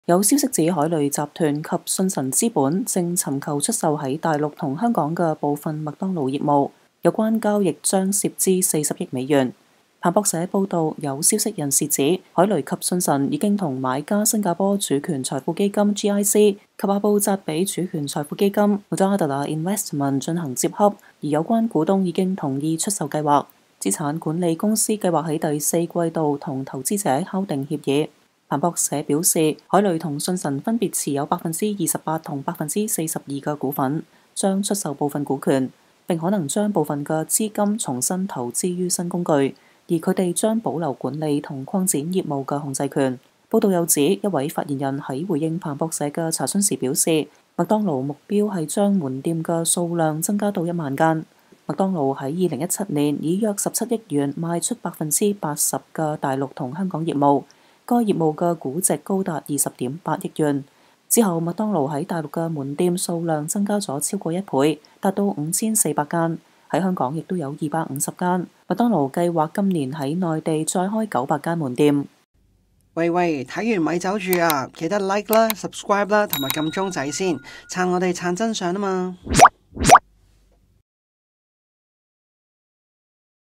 消息指，凱雷集團及信宸資本正尋求出售在大陸及香港的部份麥當勞業務，有關交易將涉資40億美元。
新紀元7月刊【跨性別的迷思】：📖現已上市🎉